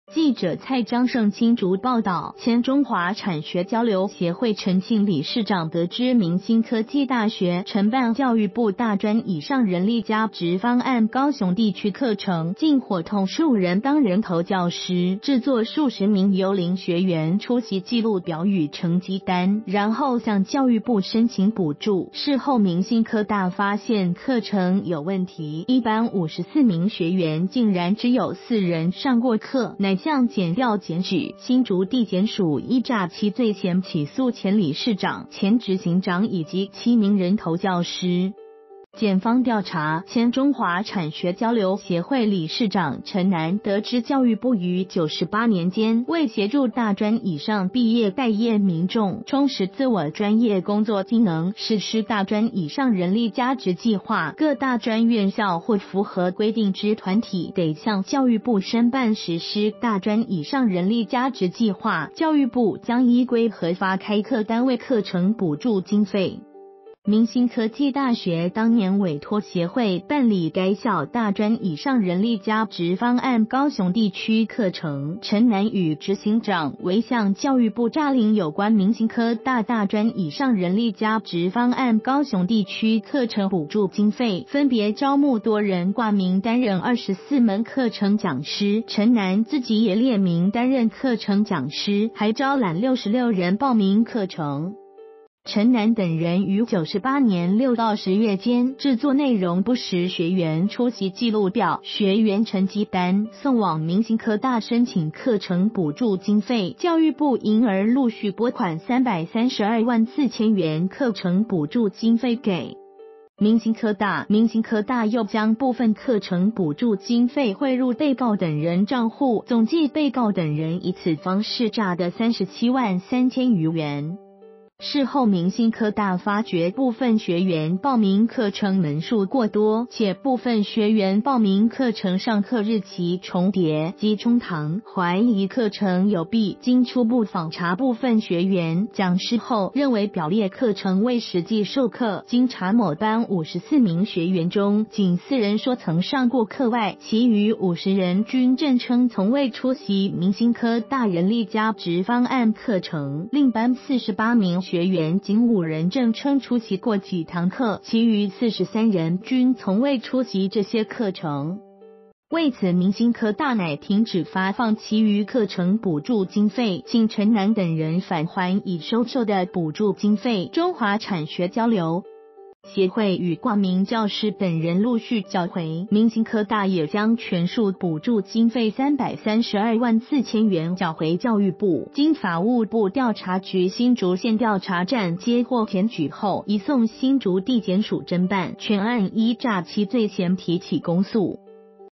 幽靈學員詐騙教部補助 前產學交流協會理事長等9人遭訴
〔記者蔡彰盛／新竹報導〕前中華產學交流協會陳姓理事長得知明新科技大學承辦教育部「大專以上人力加值方案」高雄地區課程，竟夥同數人當人頭教師，製作數十名幽靈學員出席記錄表與成績單，然後向教育部申請補助，事後明新科大發現課程有問題，一班54名學員竟然只有4人上過課，乃向檢調檢舉，新竹地檢署依詐欺罪嫌起訴前理事長、前執行長以及7名人頭教師。